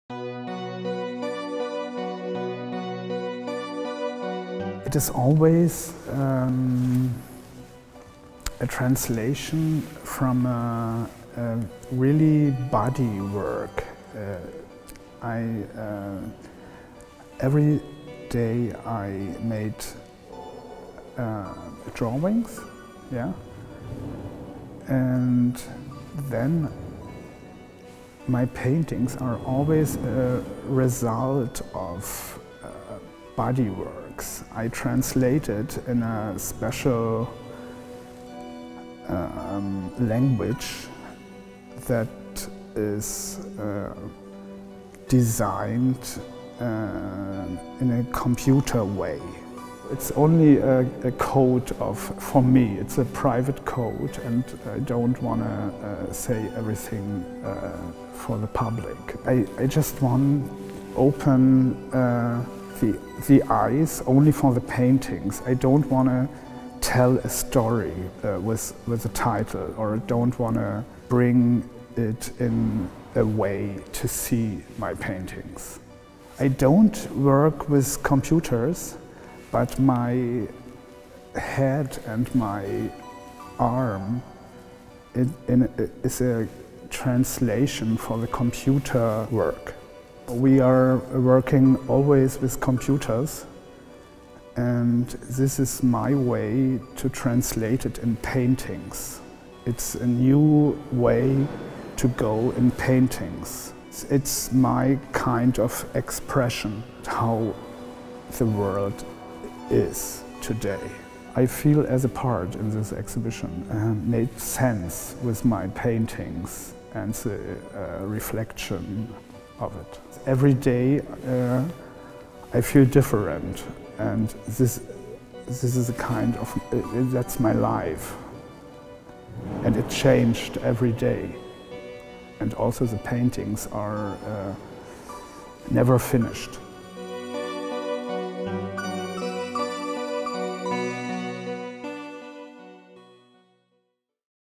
CACHE | interview | Frank Nitsche
Obrazy Franka Nitscheho nejsou – dle jeho vlastních slov – nikdy dokončené. Je možné, že i ve vaší osobní mezipaměti nadále pokračují procesy reflektování výstavy v Galerii Rudolfinum, která 31. října skončila a jíž byly malby Franka Nitscheho součástí.

Připomeňte si dílo německého umělce, který v posledních dvaceti letech výrazně reformoval současné německé malířství, v rozhovoru s autorem. Rozhovory se zbylými umělci i fotografie z výstavy naleznete na našich sociálních sítích.

Kurátor: Petr Vaňous

Frank Nitsche’s paintings are – in his own words – never finished. It is possible that the processes of reflecting on the exhibition in Galerie Rudolfinum, which ended on October 31 and of which Frank Nitsche’s paintings were a part, continue in your personal memory cache.

Recall the work of this German artist who has significantly reformed contemporary German painting of the last twenty years in an interview with the author.

Curator: Petr Vaňous